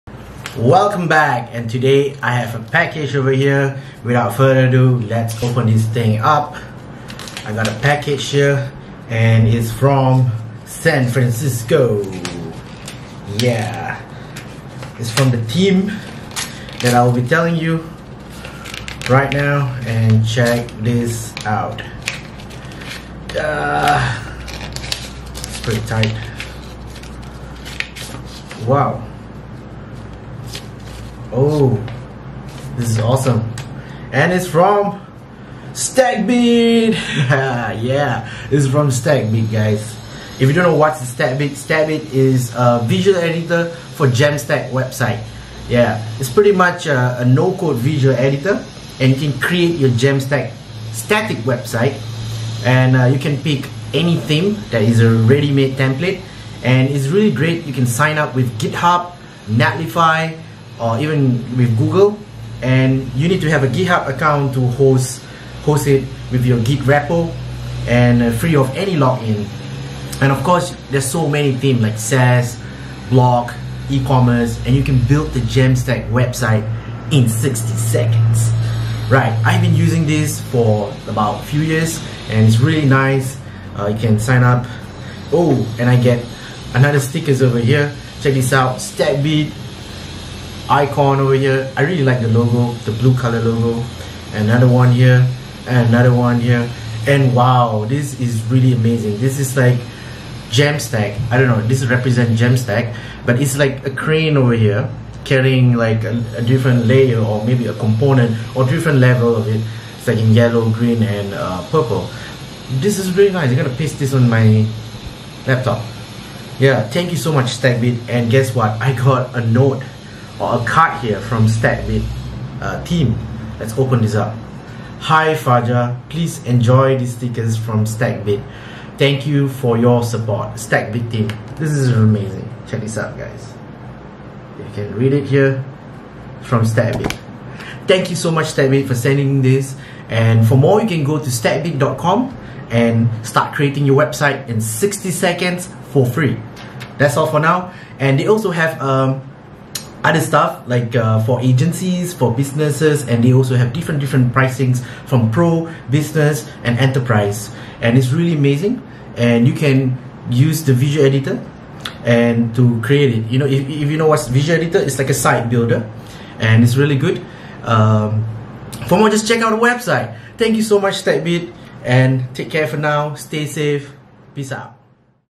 📦 Unboxing Stackbit.com stickers!
This brightens up my day! Thank you team from San Francisco for sending this sticker swag & this meaningful card. 🥰✨ I’ve been a fan of Stackbit and fall in love with JAMstack.

You can build a no-code JAMstack site with the visual editor in just 60secs. Pick your theme and make a landing page, blog, E-commerce, SaaS. I’ve made plenty of sites with Stackbit and I loving it.

💜 IRL: Vlogs, Travels, Food, Drinks, People, Places, Community
Camera Tools & Equipment
☕ Let's have a coffee chat